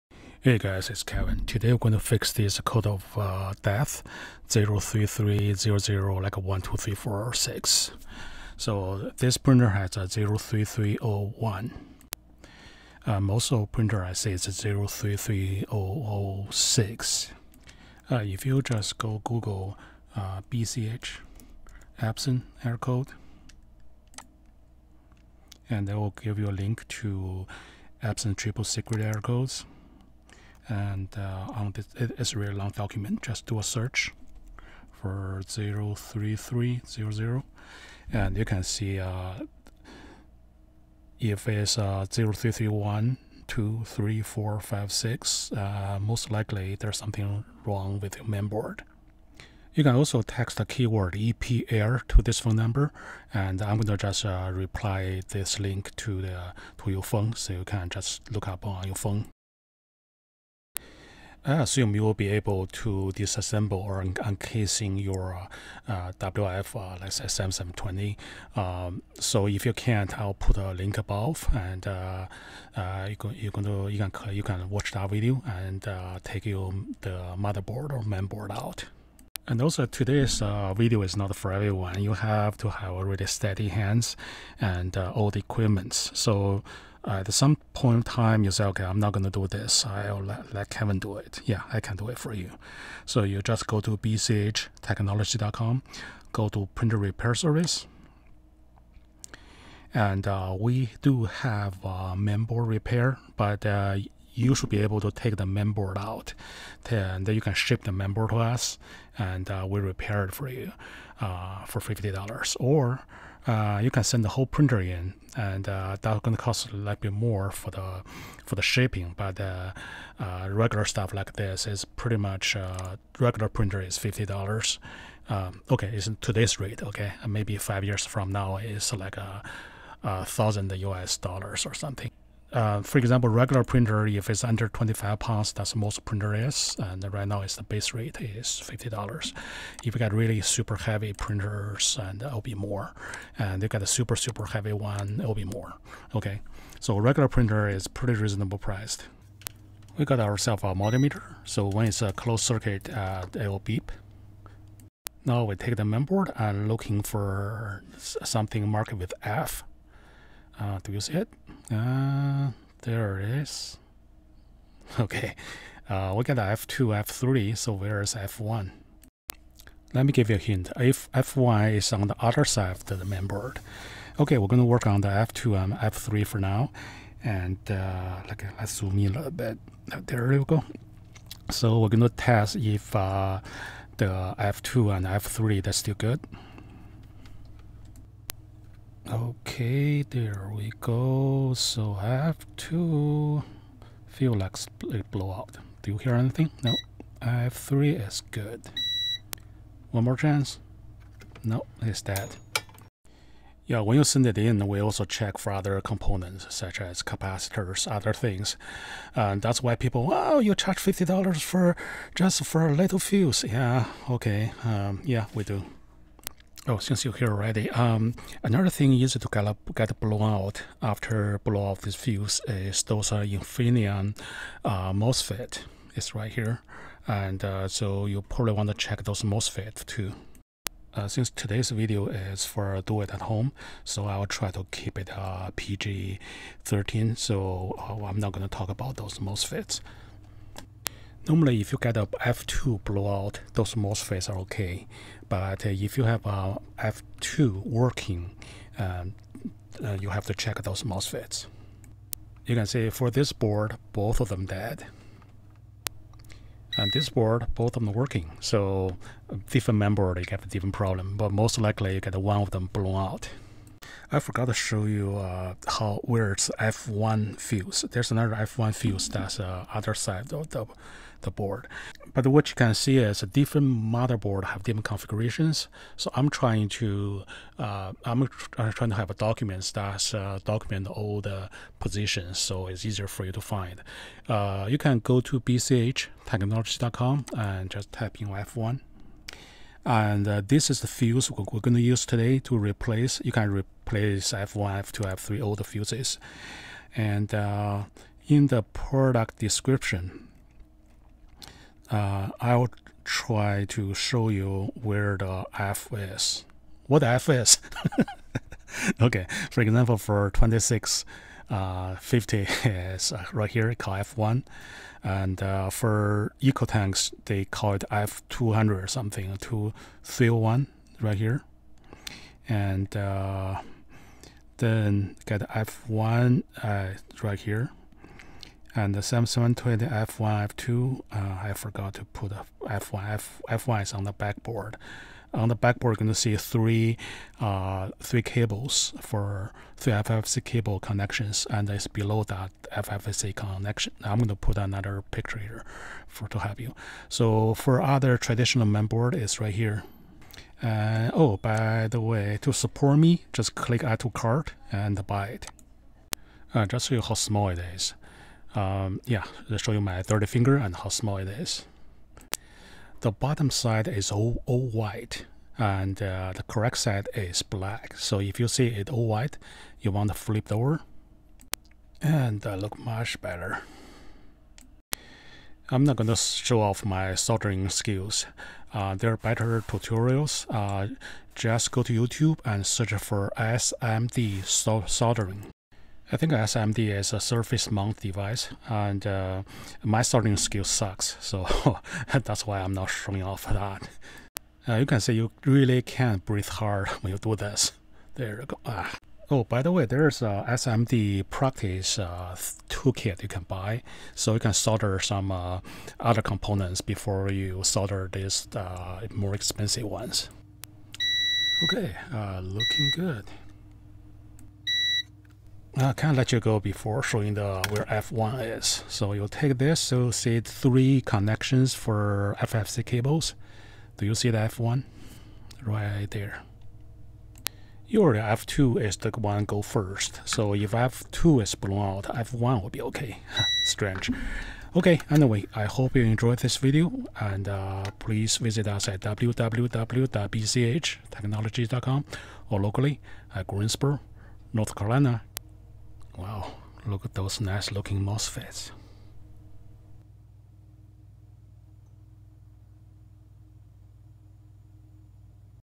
Fix "Epson Code of Death" - Error 031006 033006 or alike
If you had a cartridge leak or did a lot of printhead cleanings, you may encounter the famous "code of death" by Epson. Some of those are related to the mainboard failure. Today we demonstrate how to troubleshoot and repair the mainboard for an Epson WF-7720.